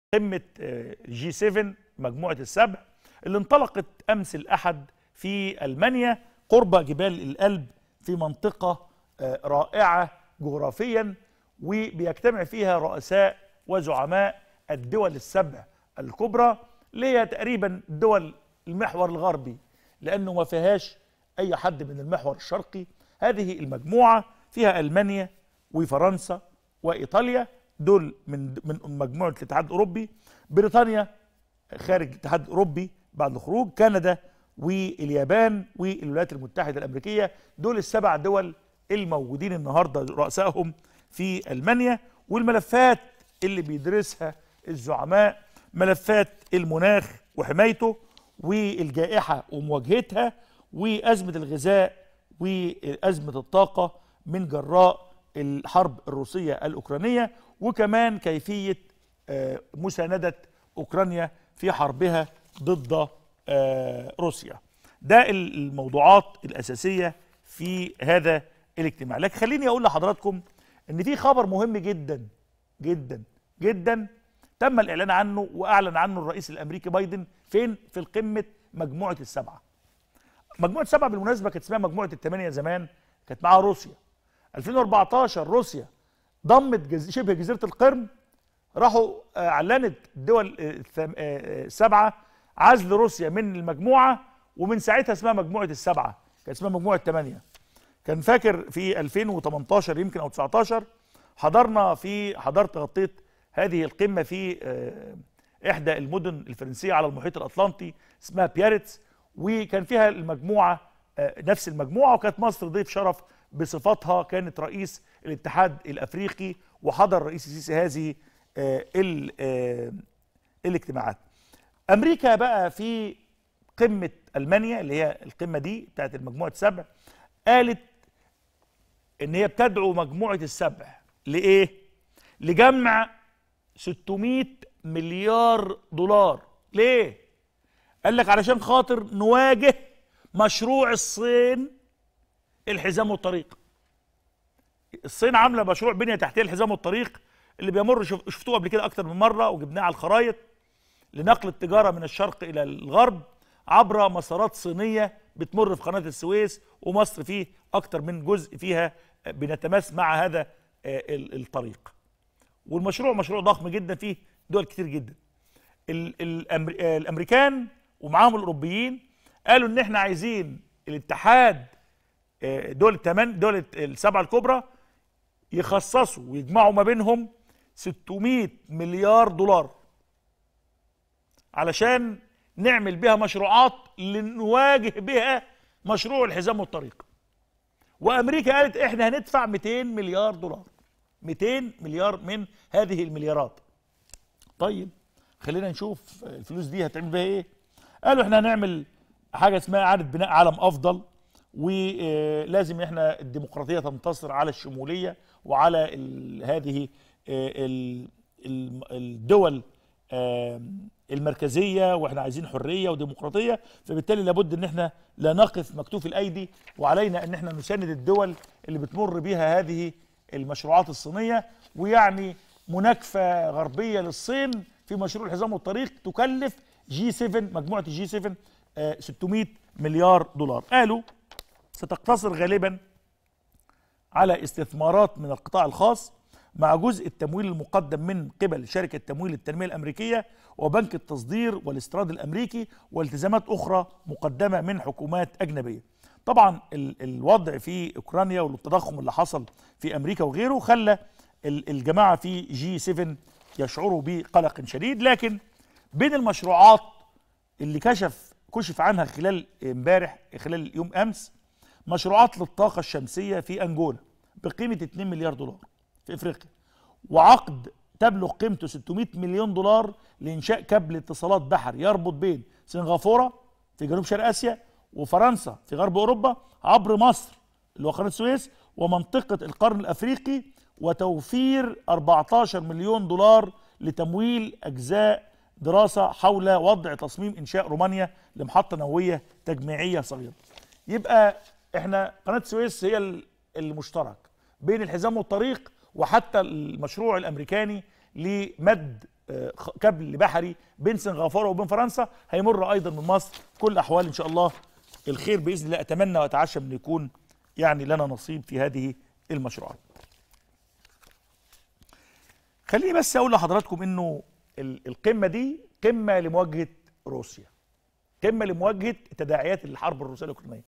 ما الملفات التي تحدثت عنها مجموعة الـ7 وما هي خطة تطوير البنية التحتية العالمية؟الديهي يكشف التفاصيل
• تابعوا الصفحة الرسمية للإعلامي نشأت الديهي على الفيسبوك ➭